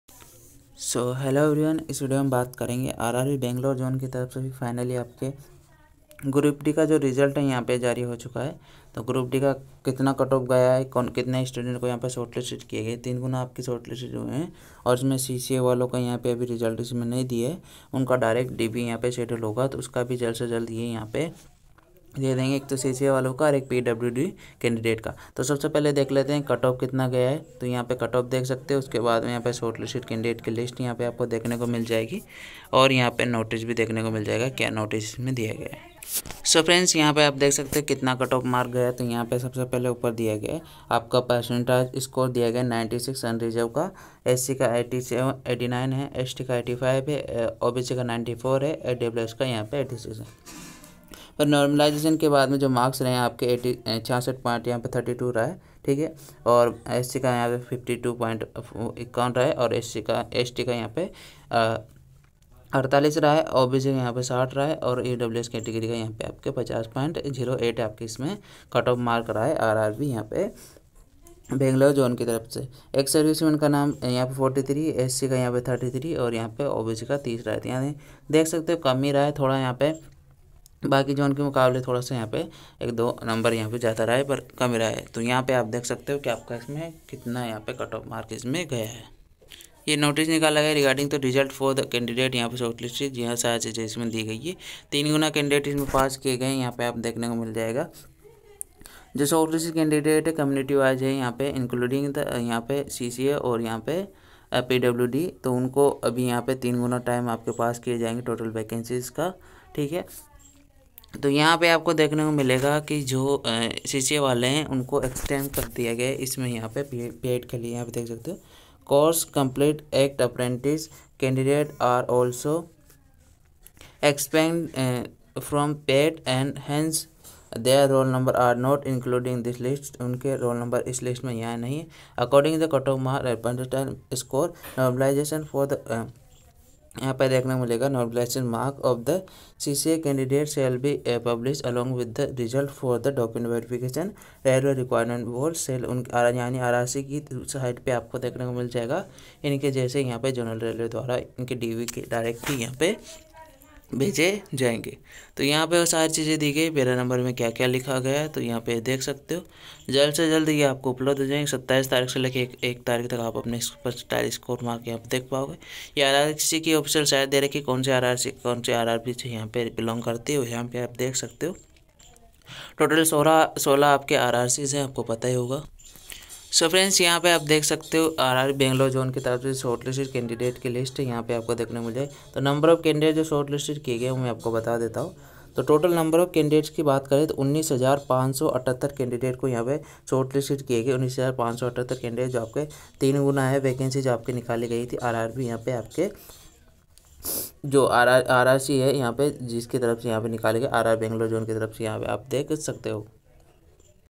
RRC group D result Bangalore zone | RRC Group D Bangalore final cut off mark For PET EXAM 2022
RRC GROUP D Cut-Off makrs for PET

Candidates shortlisted for PET to be conducted by RRC/SWR